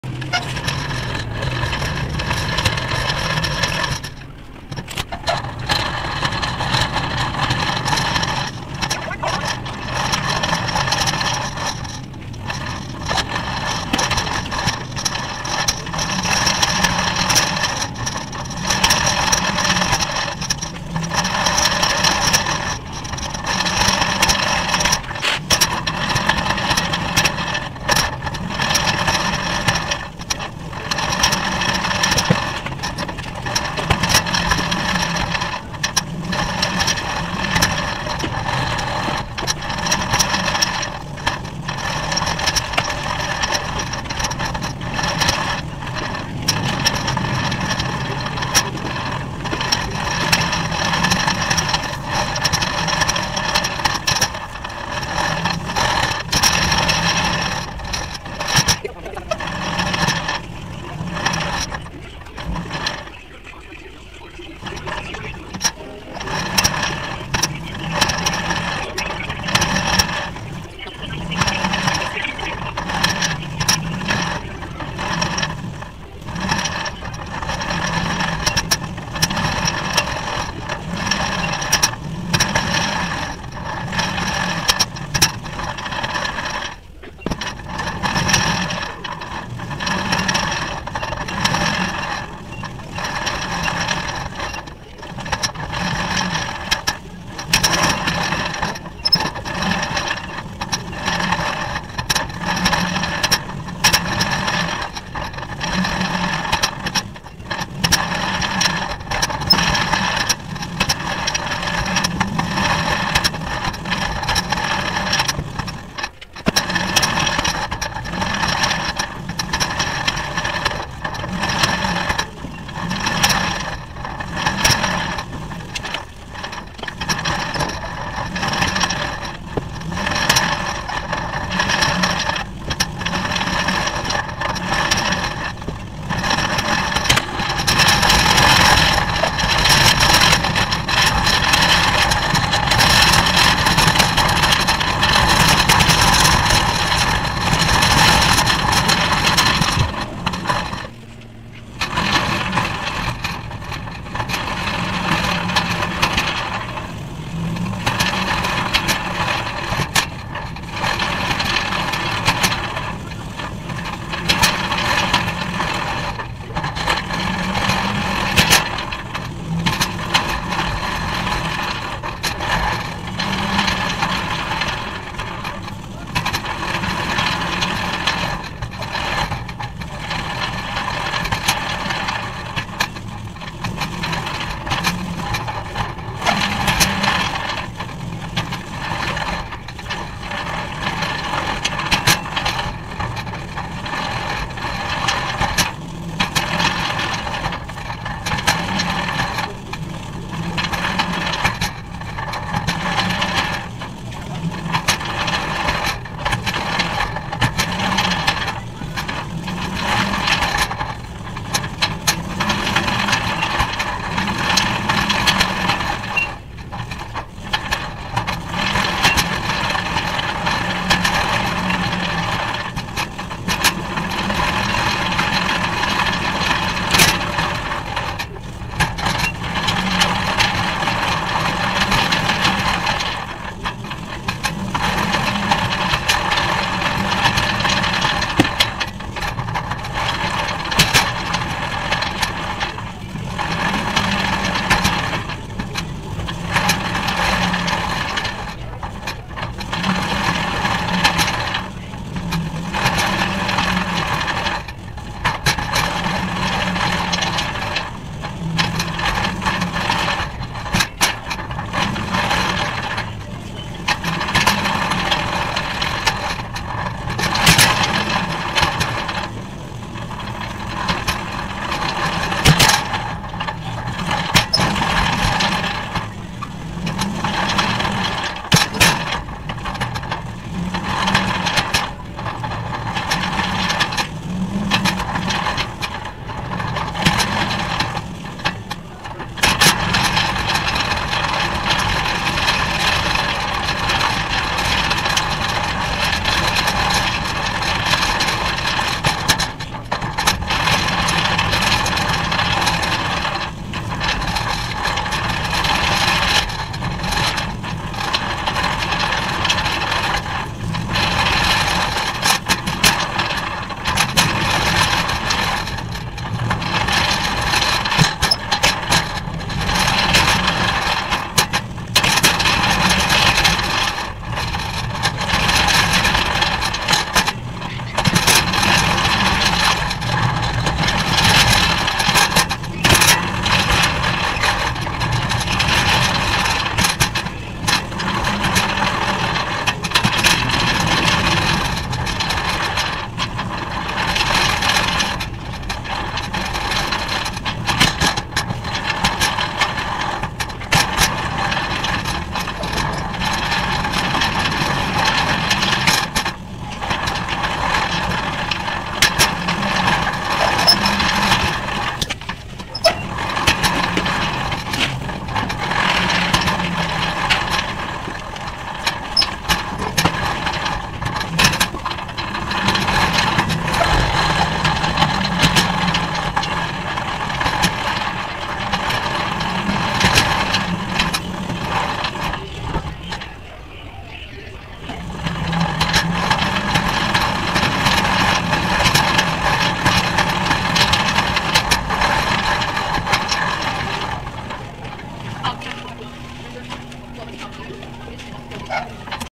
Sports HD DV Body Cam Test On A Shopping Cart Entire Store
This Was My First Time Public Test With The Bodycam Attached To The Shopping Cart. Works Great. Original Video Was 40 Min Long So I Sped It Up.